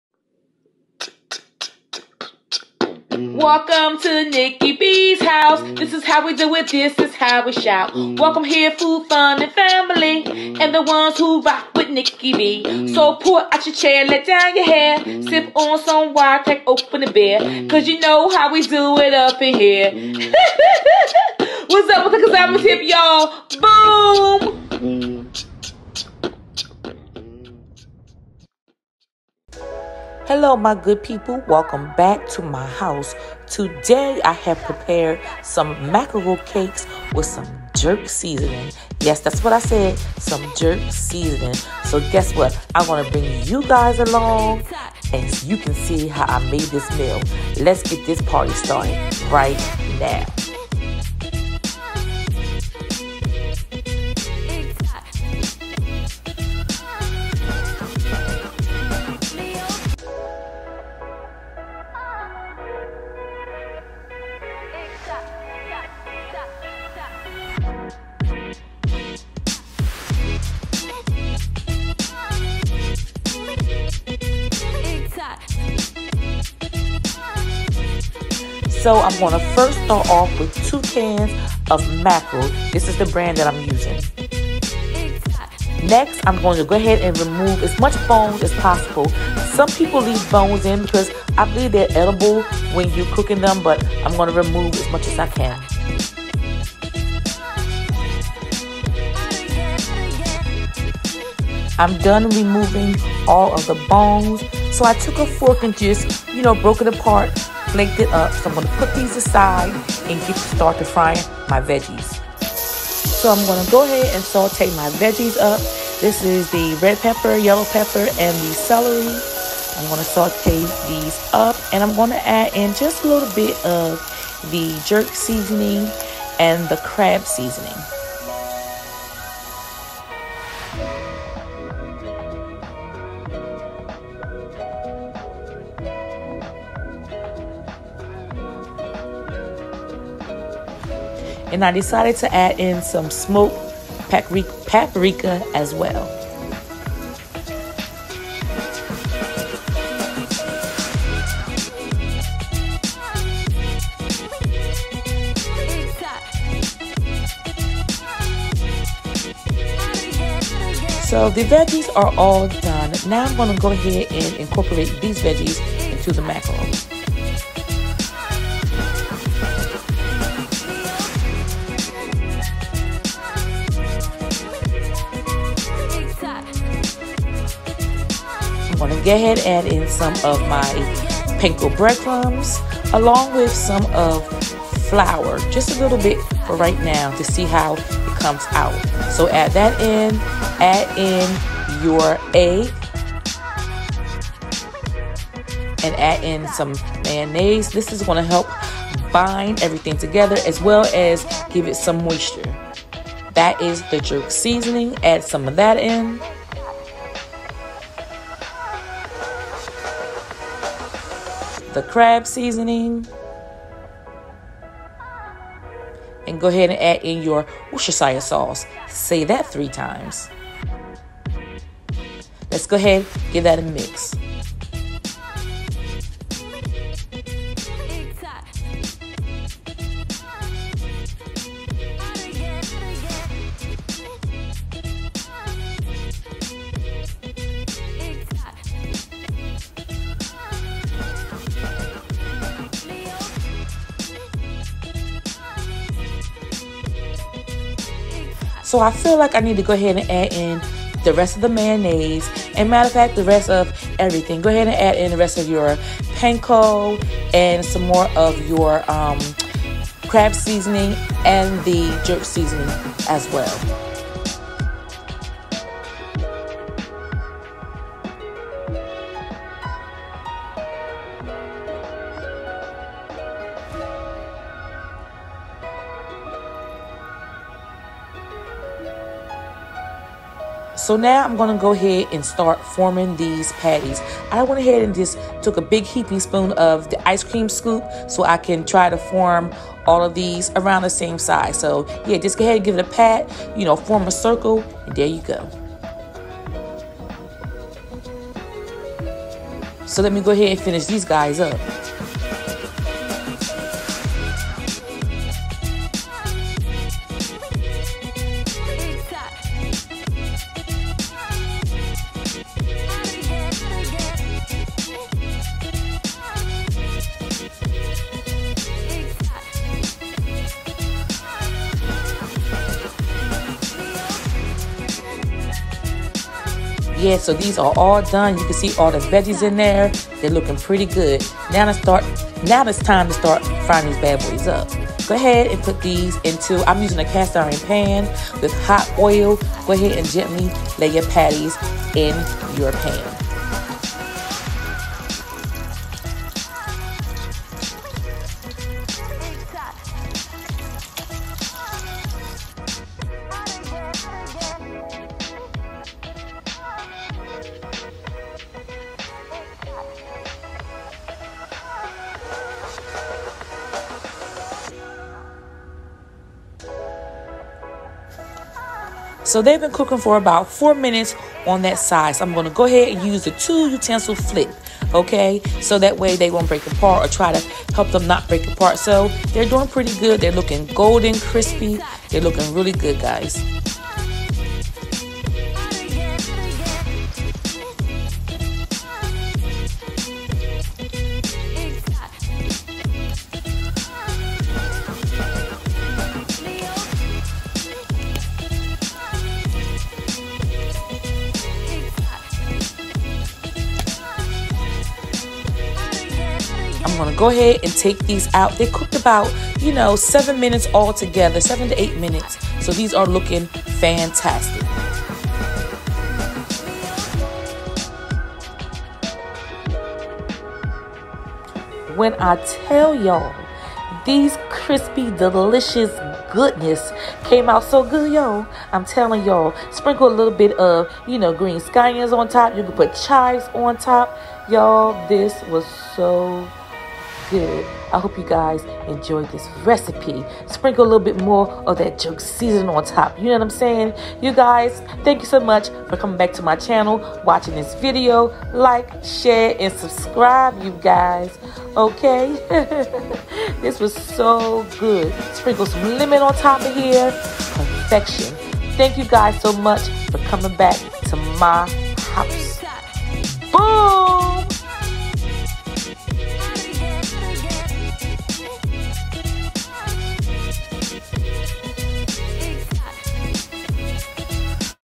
MACKEREL CAKE RECIPE WITH JERK SEASONING Mackerelcakes@nickybshouse
Happy Saturday everyone, I hope you enjoy this recipe of these delicious crispy Mackerel cakes, have a wonderful day peace and blessings🙏🏽 🥰❤️